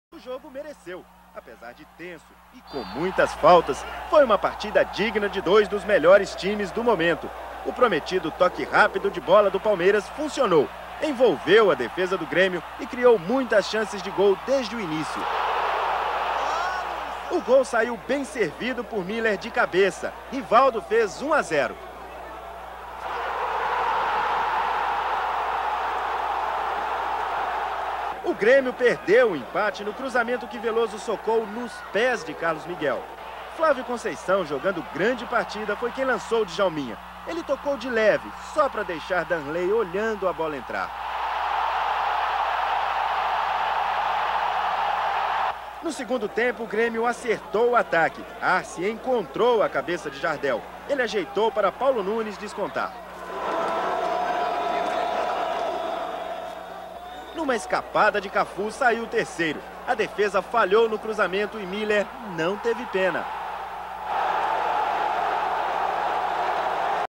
Palmeiras 3x1 Gremio - Copa do Brasil 1996
29/Maio/1996
  Copa do Brasil - Semifinais
  Local: Parque Antártica (São Paulo)  Árbitro: Wilson Souza de Mendonça (PE)
  Cartão Amarelo: Luizão, Cléber, Dinho, Luís Carlos Goiano, Djalminha e Müller.
  Gols: Rivaldo 35 e Djalminha 47 do 1º Tempo; Paulo Nunes 9 e Müller 34 do 2º Tempo.
  Palmeiras: Velloso, Cafu, Sandro Blum, Cléber e Júnior (Cláudio); Amaral, Flávio Conceição (Galeano), Rivaldo e Djalminha; Müller e Luizão (Elivélton). Técnico: Vanderlei Luxemburgo.
  Grêmio: Danrlei, Arce, Rivarola, Luciano e Carlos Miguel; Dinho (Émerson), Luís Carlos Goiano,  João Antônio e Aílton (Rodrigo); Paulo Nunes (Zé Alcino) e Jardel. Técnico: Luiz Felipe Scolari.